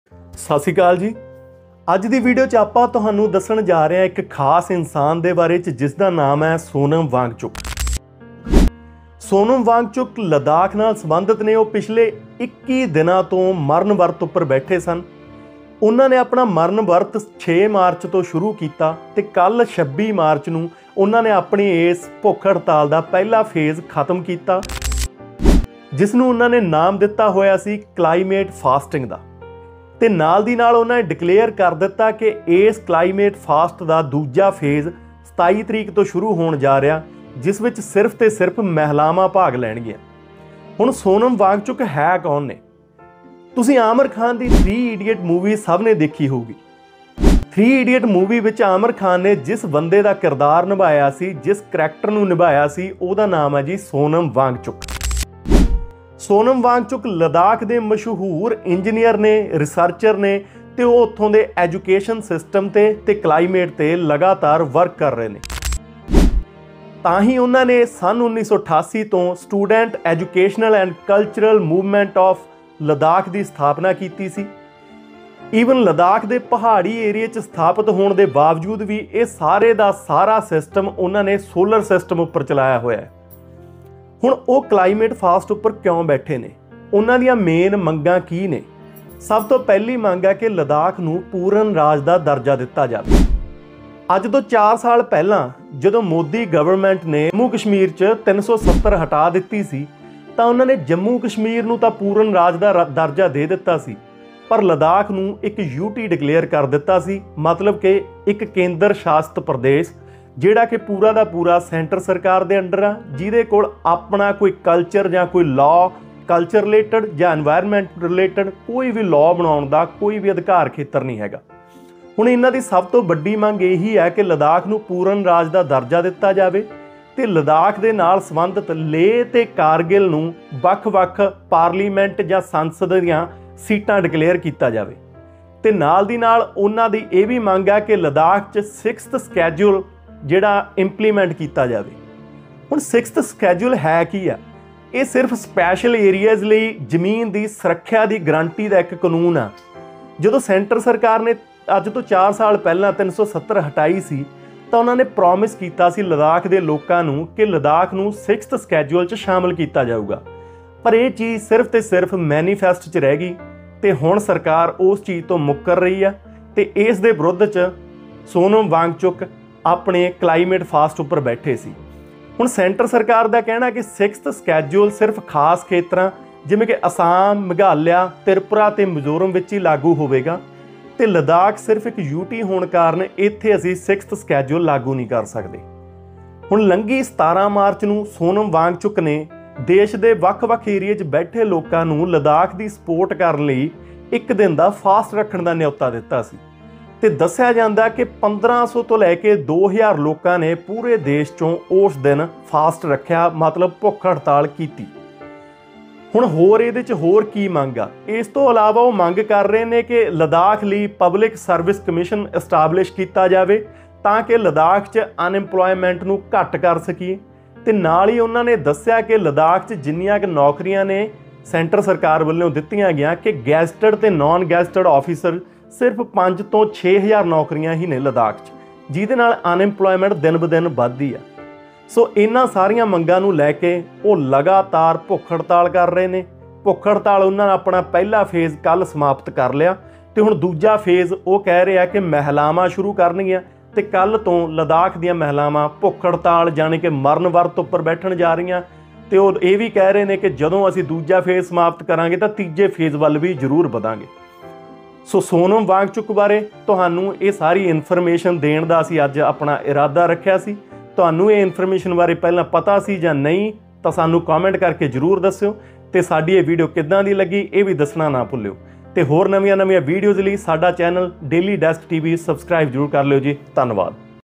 ਕੌਣ ਹੈ ਸੋਨਮ ਵਾਂਗਚੂ !! 21 ਦਿਨਾਂ ਦਾ CLIMATE FAST | ARTICLE 370 | Statehood for Ladakh | DAILY DESK TV
We welcome all the viewers on our channel  @TheDailyDesktv

In this video you can watch information about SONAM WANGCHUK. He is a renown Engineer and Climate Activist who was sitting on CLIMATE FAST for the people of LADAKH.
His main demand is full STATEHOOD FOR LADAKH and implementation of 6th SCHEDULE which was promised by BJP GOVERNMENT after removing ARTICLE 370 from J&K.

SONAM WANGCHUK
CLIMATE FAST
STATEHOOD FOR LADAKH
ARTICLE 370